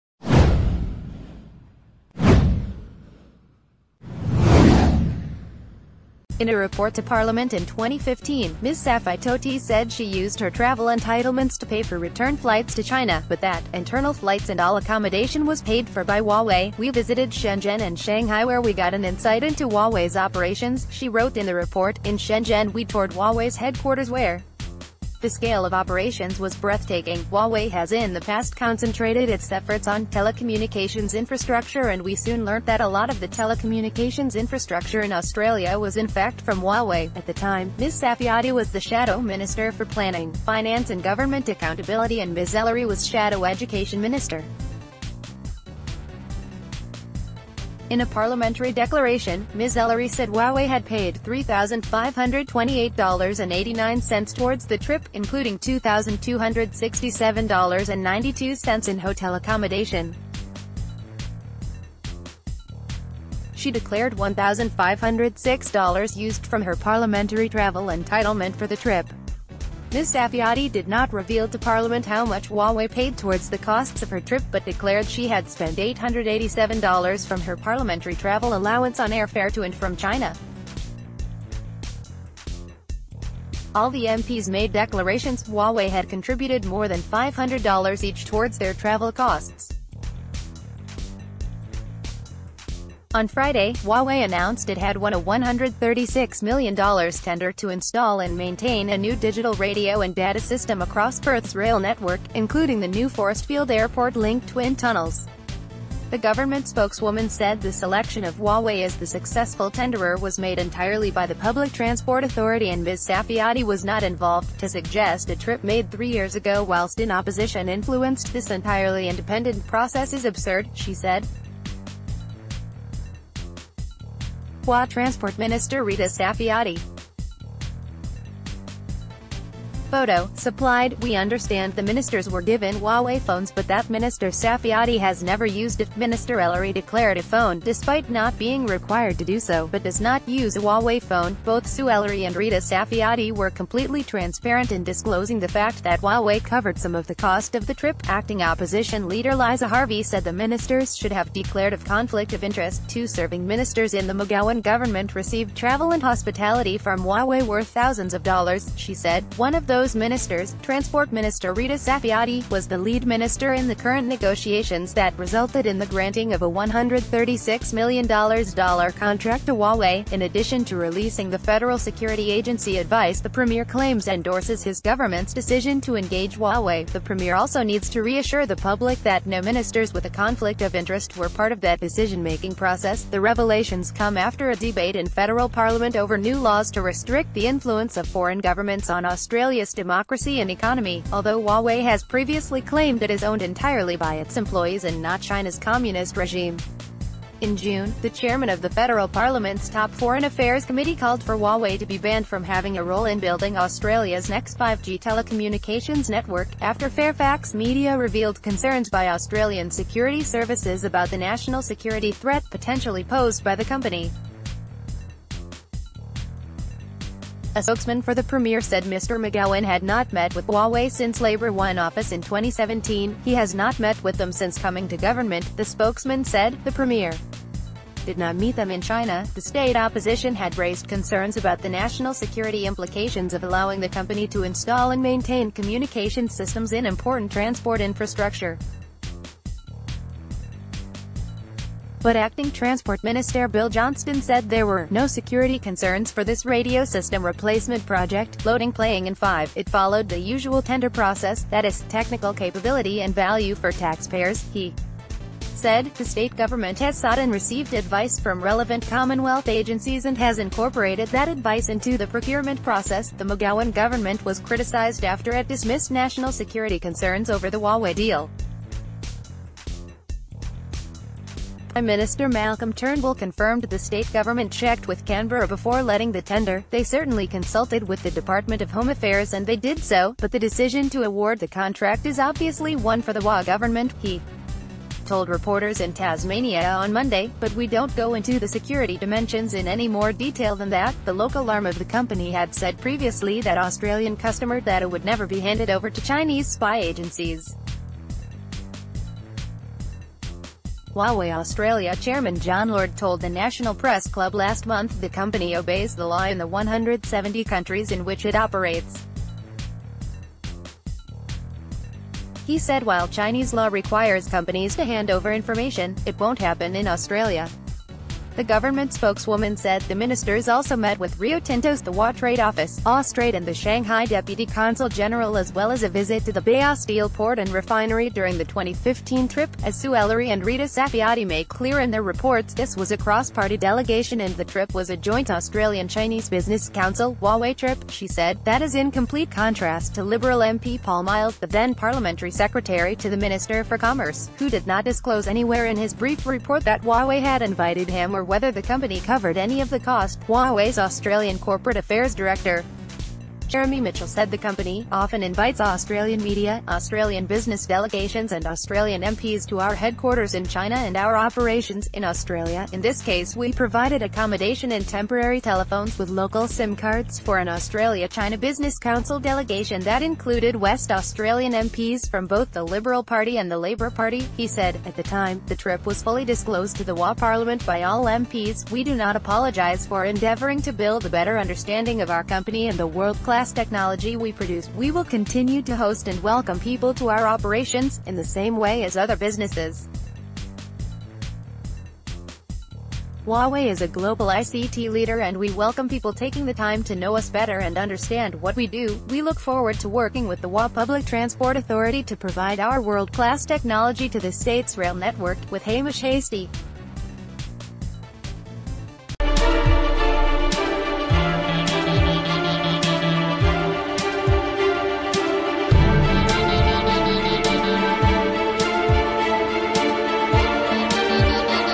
'Breathtaking': Huawei paid for MPs' China tour, gave free phones to Saffioti, Ellery
In a report to parliament in 2015, Ms Saffitoti said she used her travel entitlements to pay for return flights to China, but that "internal flights and all accommodation was paid for by Huawei"."We visited Shenzhen and Shanghai where we got an insight into Huawei's operations", she wrote in the report."In Shenzhen we toured Huawei's headquarters where the scale of operations was breathtaking."Huawei has in the past concentrated its efforts on telecommunications infrastructure and we soon learnt...